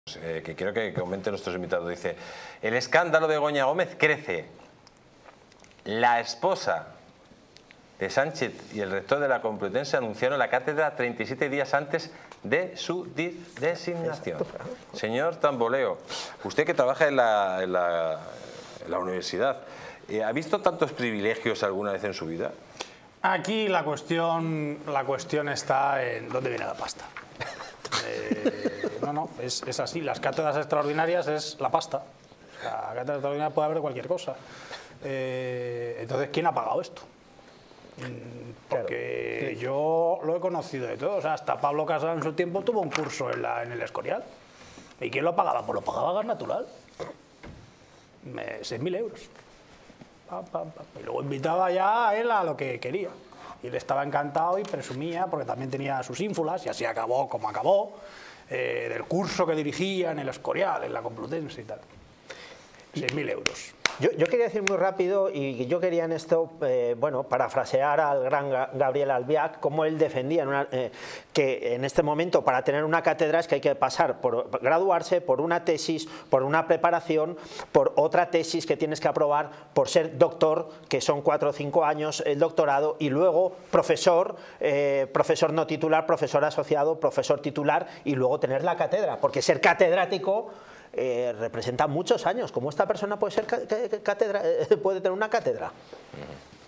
🔴La esposa de Sánchez y el rector de la UCM anunciaron la cátedra 37 días antes de su designación🔴
En este video, analizaremos el anuncio de la cátedra de la esposa de Sánchez y el rector de la UCM 37 días antes de su designación. ¡No te lo pierdas!